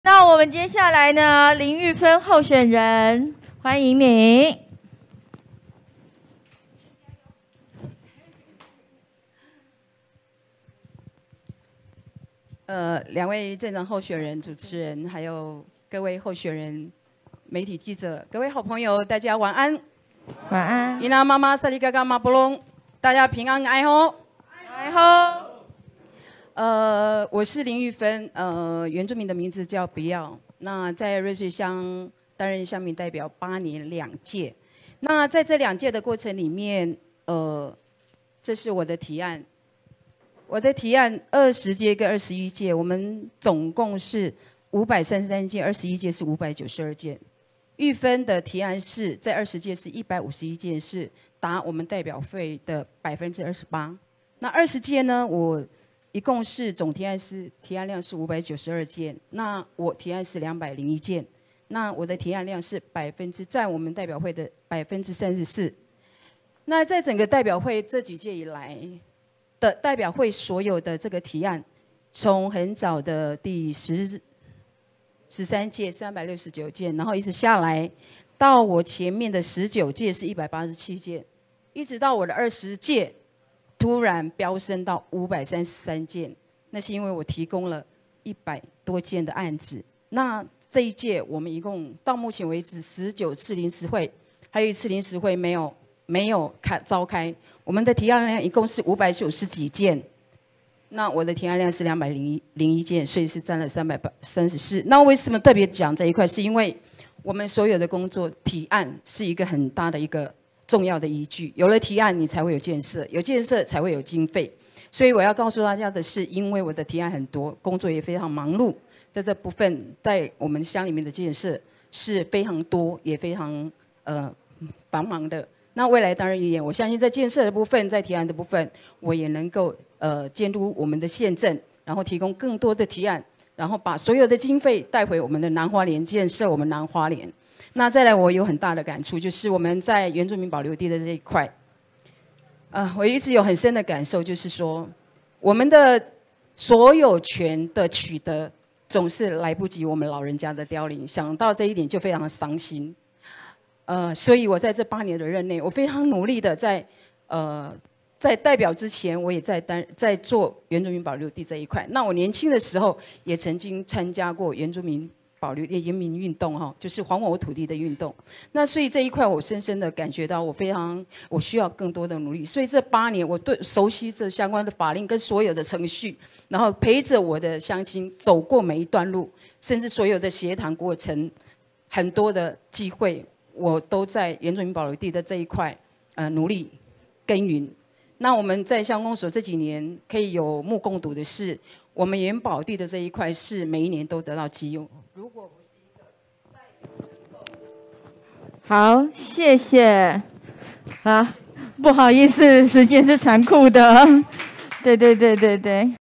林玉芬在玉里鎮民間政見發表會上的發言
瑞穗鄉擔任鄉民代表八年兩屆（20、21屆）
兩屆提案量占百分之二十八跟百分之三十四。
提案是一個重要的依據，有提案有經費，有經費才有建設。
提供更多提案經費帶回南花蓮
擔任代表前就接觸原民保留地，年輕時參與還我土地運動。原保地每年都得績優